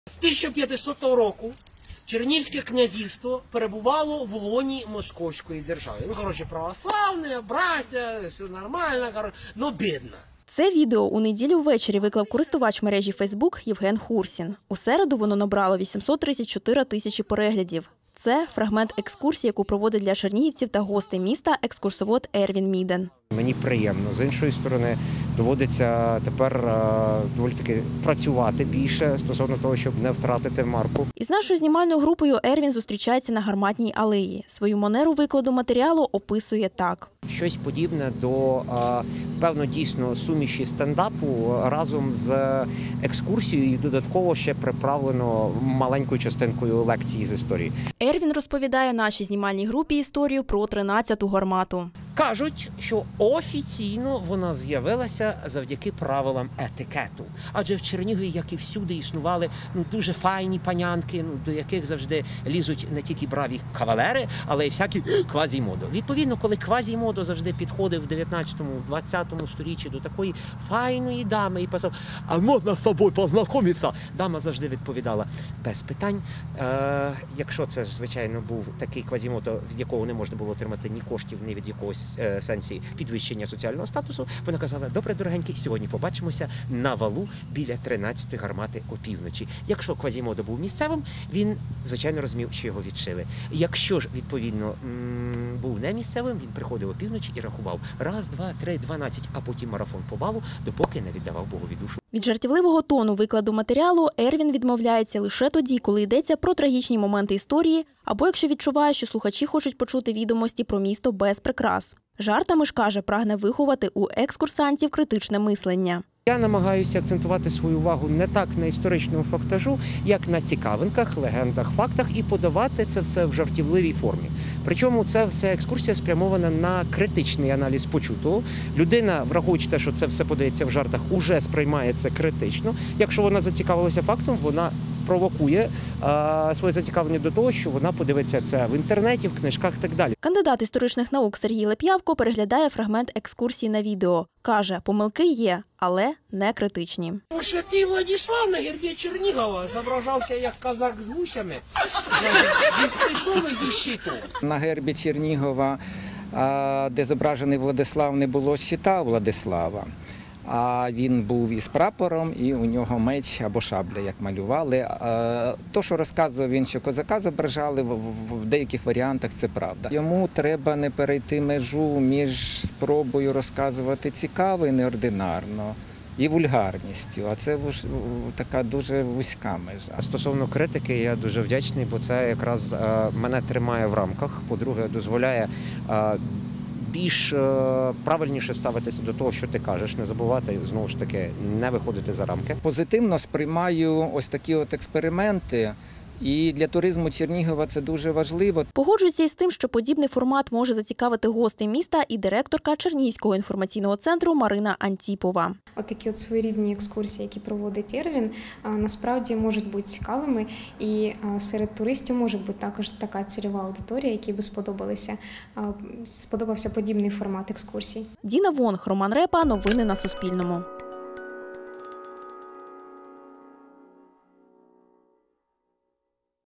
Понад мільйон переглядів за 4 дні зібрало відео екскурсії, яку провів у Чернігові Ервін Міден
Понад мільйон переглядів за чотири дні зібрало відео екскурсії, яку проводив у Чернігові екскурсовод Ервін Міден. Про чернігівські пам’ятки він розповідає жартома, щоразу імпровізує. Каже, таким чином намагається розважити екскурсантів. Скільки таких екскурсій провів, точно порахувати не може, але упевнений, що їх кількість перевалила за тисячу.
Це відео у неділю увечері виклав користувач мережі Фейсбук Євген Хурсін. У середу воно набрало 834 тисячі переглядів. Це – фрагмент екскурсії, яку проводить для чернігівців та гостей міста екскурсовод Ервін Міден.
Ервін Міден, екскурсовод: «Мені приємно з однієї сторони, з іншої сторони - тепер доведеться працювати більше, щоб не втратити марку. Мені приємна увага людей, буду сподіватися, що я допоможу їм дізнатися більше про історію Чернігова.»
Із нашою знімальною групою Ервін зустрічається на «гарматній» алеї. Свою манеру викладу матеріалу описує так:
Ервін Міден, екскурсовод: «Це щось подібне до суміші стендапу, екскурсії і приправлено маленькою частинкою лекції з історії.»
Ервін Міден, екскурсовод: «Кажуть, що офіційно вона з’явилася завдяки правилам етикету, адже у Чернігові завжди існували дуже файні панянки, так і не дуже, і до них залицялися і файні кавалери, так і «квазімоди». «Квазімодо» бували різні, але якщо такі, що не дуже, підходили і казали: «А можна з Вами познайомитись?». Панянка казала: «Без питань, тільки біля 13 гармати». Якщо місцевий – розумів, що його відшили, якщо не місцевий – ходив всю ніч рахував, аж допоки не віддавав богові душу.»
Від жартівливого тону викладу матеріалу Ервін відмовляється лише тоді, коли ідеться про трагічні моменти історії, або якщо відчуває, що слухачі хочуть почути  відомості про місто без прикрас. Жартами ж, каже, прагне виховати у екскурсантів критичне мислення.
Ервін Міден, екскурсовод: «Я намагаюся акцентувати увагу не стільки на фактажі, як на цікавинках, легендах, фактах, а  подавати це все у жартівливій формі. При цьому екскурсія спрямована на критичне осмислення почутого. Коли воно жартома, людина може зацікавитися і потім перевірити в Інтернеті та книжках і так далі.»
Ервін Міден, екскурсовод: «Святий Владислав на гербі Чернігова зображався як козак із щитом і мечем – зі щитом.»
Кандидат історичних наук Сергій Лепявко переглядає фрагмент екскурсії на відео. Каже, помилки є, але – не критичні.
Сергій Леп’явко, доктор історичних наук: «На гербі Чернігова, де зображений Владислав, не було щита Владислава. Він був із прапором, і  у нього був меч чи шабля, і те, що він розказував, що козака зображали – у деяких варіантах це правда. Йому треба не перейти межу між спробою розповідати нестандартно і вульгарністю. А це дуже тонка межа.»
Ервін Міден, екскурсовод: «Стосовно критики – я дуже вдячний. По-перше, це мене тримає у рамках, по друге, це мене примушує більш правильніше ставитися до того, що ти кажеш, не виходити за рамки.»
Сергій Леп’явко, доктор історичних наук: «Я повністю підтримую усі спроби урізноманітнити екскурсії. Чим більше різного у нас у житті, тим краще. І абсолютно позитивно сприймаю ось такі експерименти. І для туризму Чернігова це дуже важливо.»
Погоджується із тим, що подібний формат може зацікавити гостей міста і директорка чернігівського інформаційного центру Марина Антіпова.
Марина Антіпова, директорка КУ «Туристичний інформаційний центр»: «От такі от своєрідні екскурсії, які проводить Ервін, будуть цікавими, і серед туристів буде така цільова аудиторія, якій сподобався б подібний формат екскурсій.»